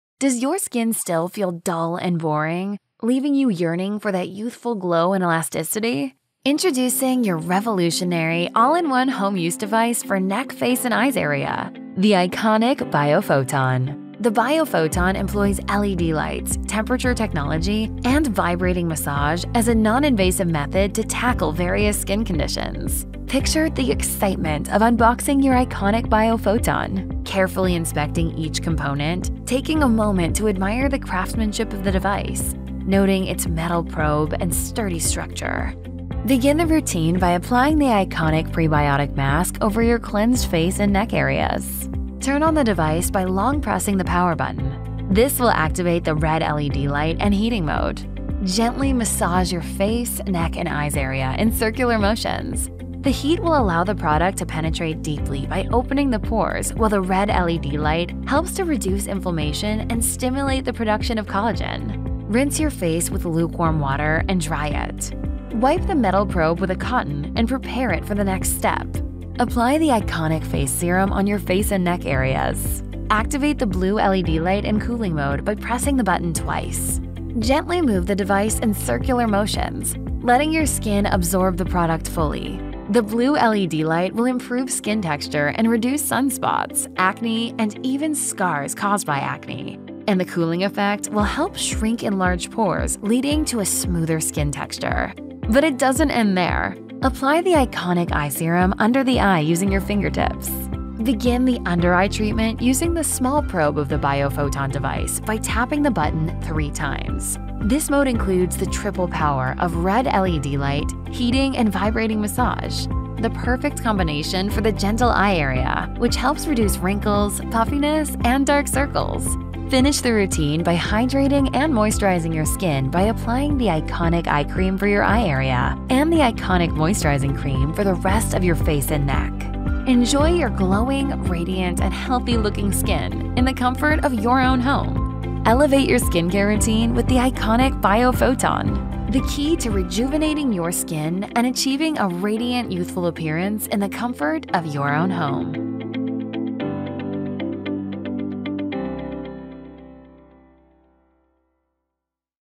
Iconique Biophoton
Fight back Skin Dullness and Improves Collagen production.

The Biophoton is your new spa expert at home.
With revolutionizing technology using Photons or LED lights as non-invasive procedure to treat inflammation and acne.
The use of different colored LED lights have been subject to many studies most notably by NASA about it being a possible way to heal wound and an alternative to chemotherapy.
Through the use of LED lights, the skin absorbs the light deep into the layers of the skin accelerating cell biochemical circulation and stimulates the production of the collagen making the skin elastic and youthful-looking.

Benefits:

Improve the skin’s blood circulation or reduce swelling.
Reduces inflammation and acne.
Skin toxin elimination, shrinking of pores.
Eliminate swelling, improve skin tone.

Or